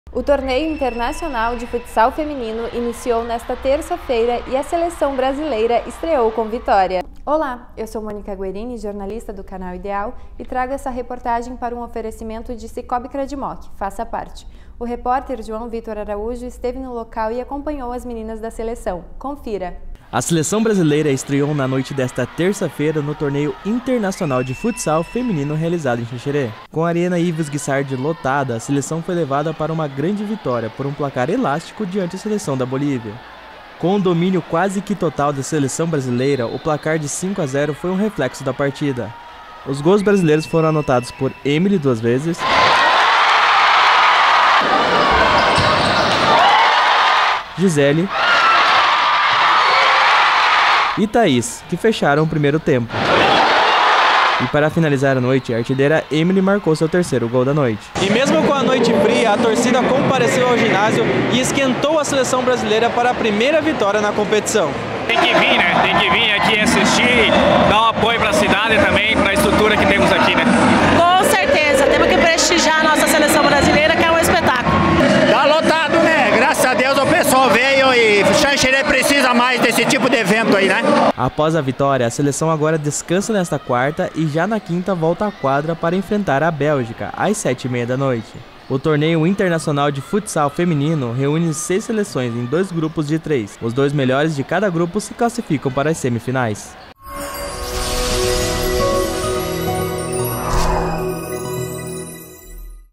Seleção brasileira estreia com goleada no Torneio Internacional de Futsal Feminino, em Xanxerê
A Seleção brasileira entrou em quadra na noite desta terça-feira (09) e estreiou com vitória no Torneio Mundial de Futsal Feminino. Arena Ivo Sguissardi ficou lotada e a vibração da torcida levou o Brasil para a vitória por 5 a 0.

Foram 4 gols no primeiro tempo. Emilly abriu o placar, Giselli e Taís fecharam o primeiro tempo e para finalizar a noite, a artilheira, Emilly marcou seu terceiro gol da noite. Brasil 5 a 0 na estreia contra a Bolívia.

A equipe brasileira volta a quadra nesta quinta-feira (11), diante a Bélgica.